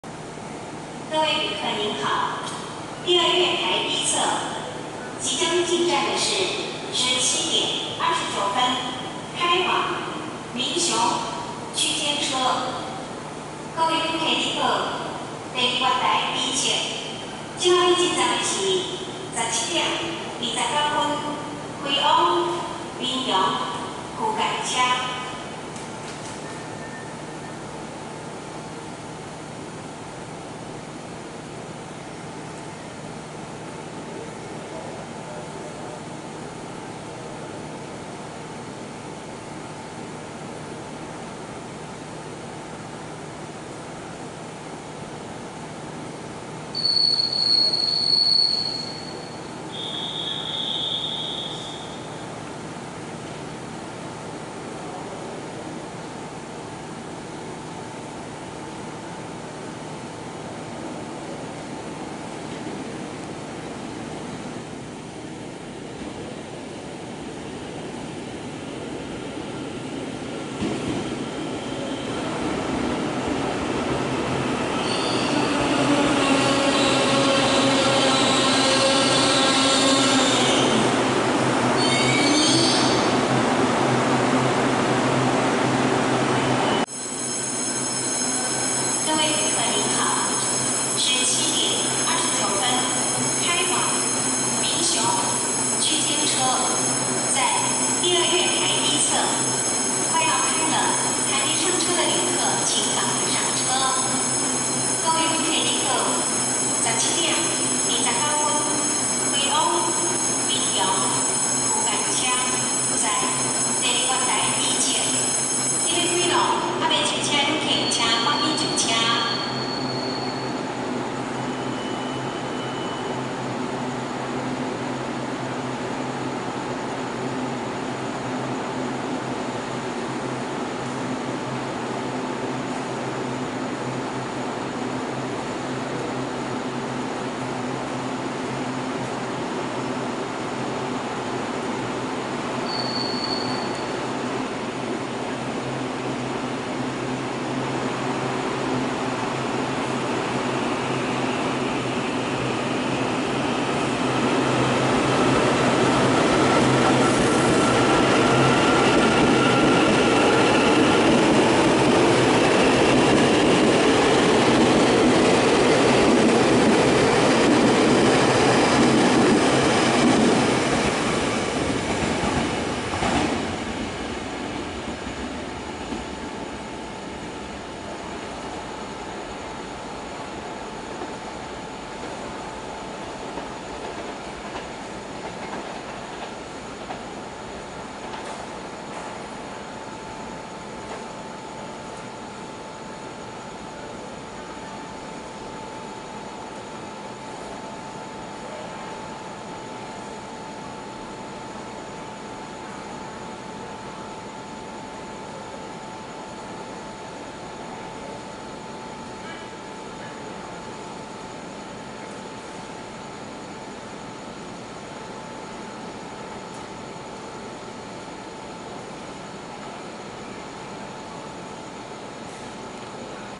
2020-09-15 新左營車站EMU800(839+840)2B進出站
區間車  3242車次  潮州開往民雄